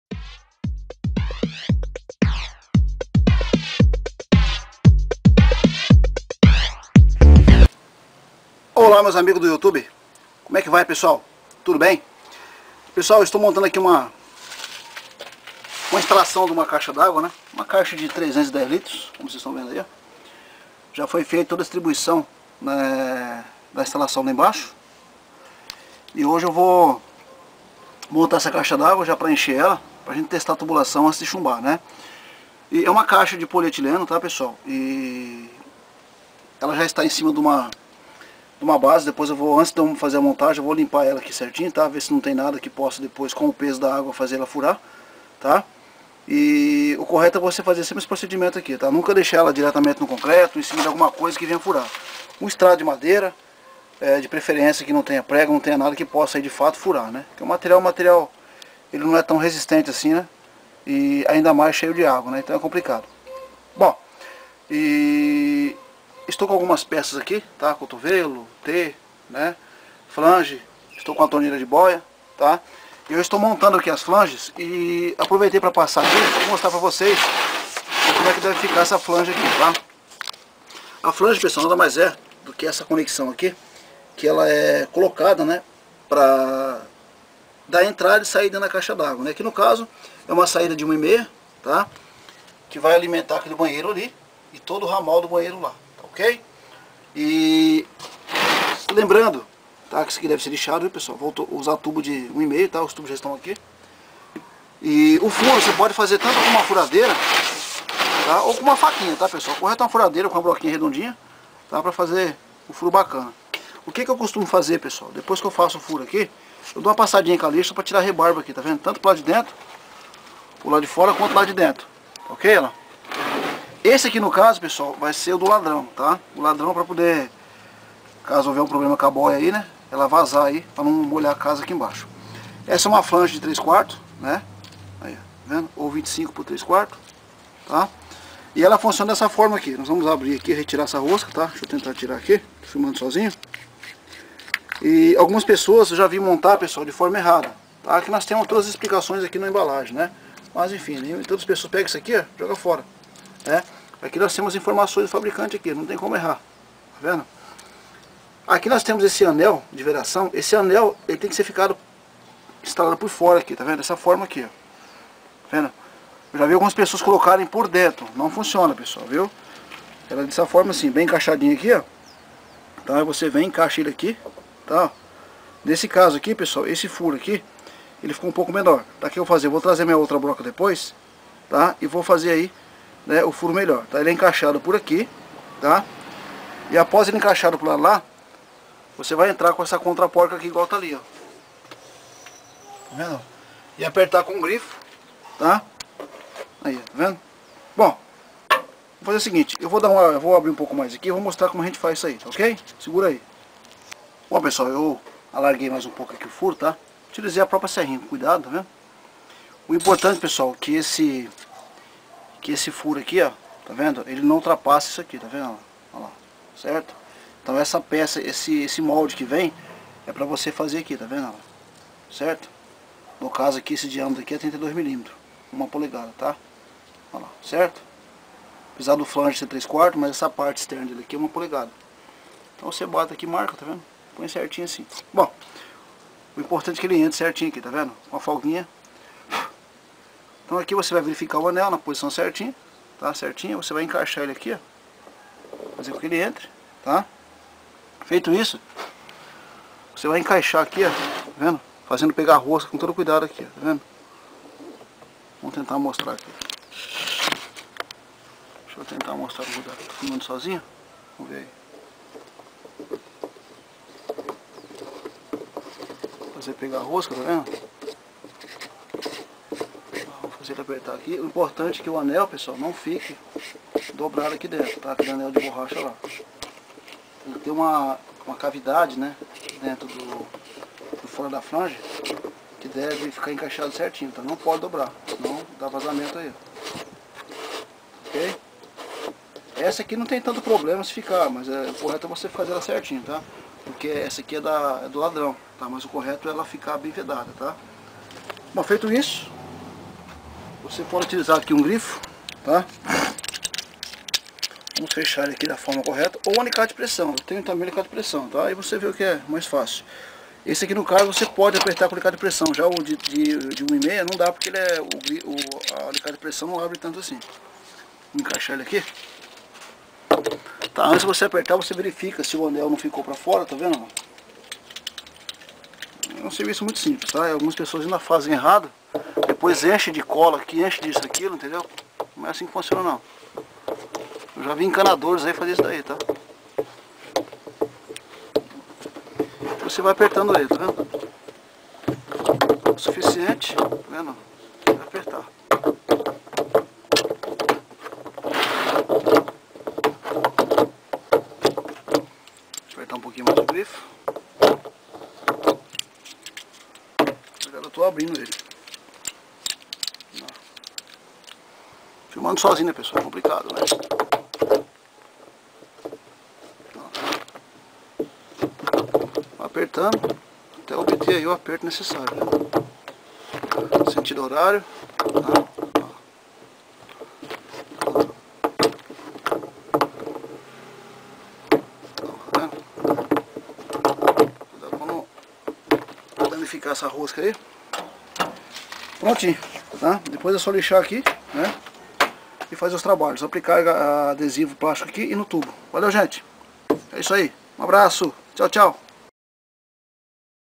vejam como devemos fazer a instalação correta de flanges em caixas d' águas de polietileno
►►Informações do vídei◄◄
Neste vídeo você vai aprender como devemos fazer a instalação correta de flanges em caixas d' águas de polietileno de forma simples e funcional.

CONTATO
Skype: ajmconstrucoesreformas

Melhores downloads gratuitos!!